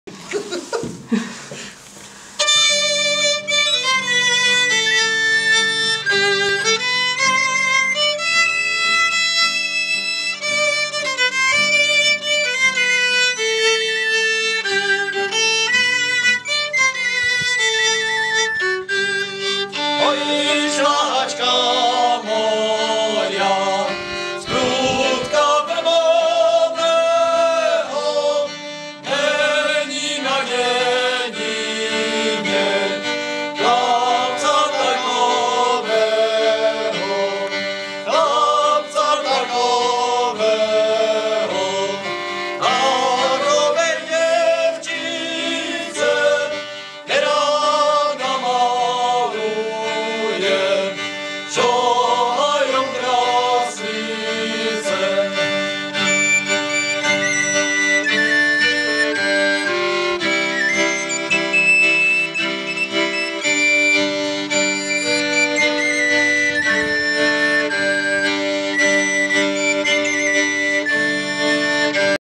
Milý závěr velkonočního pondělí
Na závěr velikonočního pondělí mne teď před chvilkou takto potěšili zpěváci a muzikanti z Ostrožské Lhoty. Hoši, moc děkuji!!!